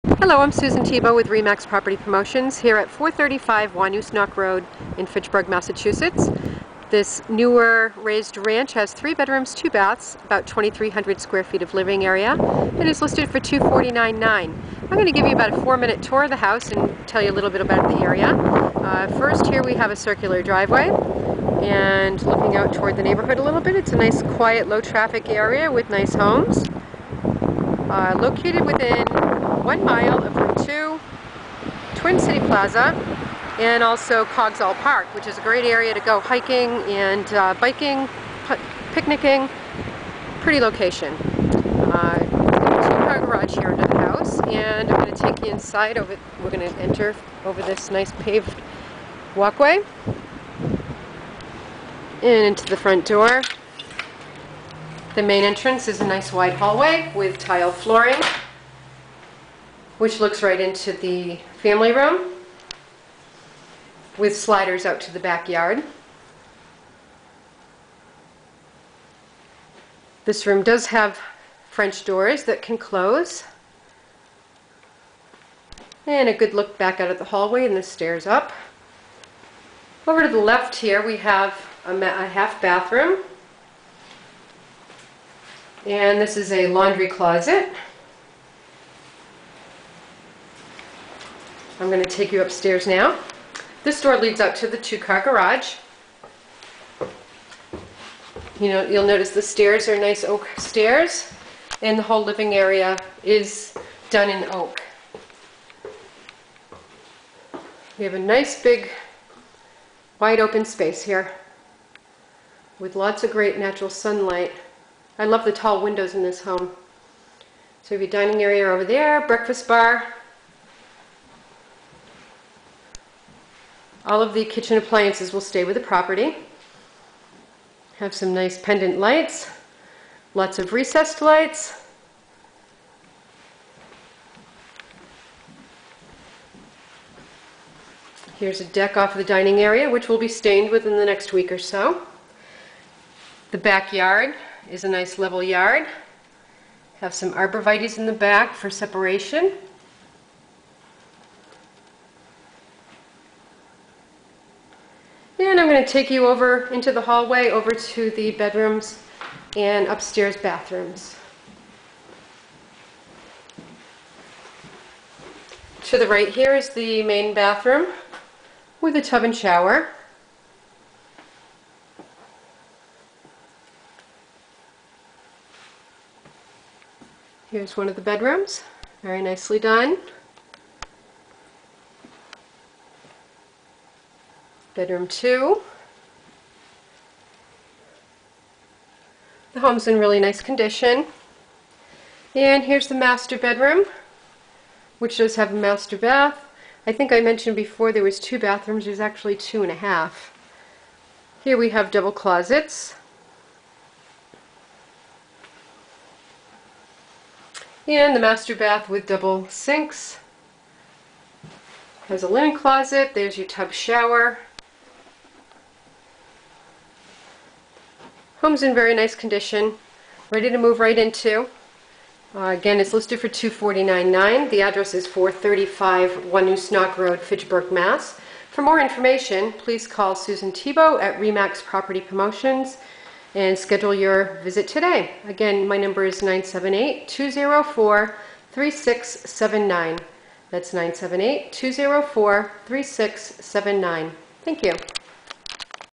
SOLD- 435 Wanoosnoc Rd., Fitchburg, MA 01420
SOLD- $240,000
This 3 BR, 2 1/2 bath raised ranch has 2280 SF of living area and a 2 car garage.  Circular drive, 1/3 acre level lawn on a quiet low traveled street within 1 mile to Rt 2, Twin City Plaza and Coggshall Park.  Big open concept kit/LR/DR with hardwood floors and big center island.  Sliders lead to deck.  Spacious tiled foyer leads to family room with French doors and sliders.  If you are looking for a newer spacious home, you'll want to come see this one.  Please call today.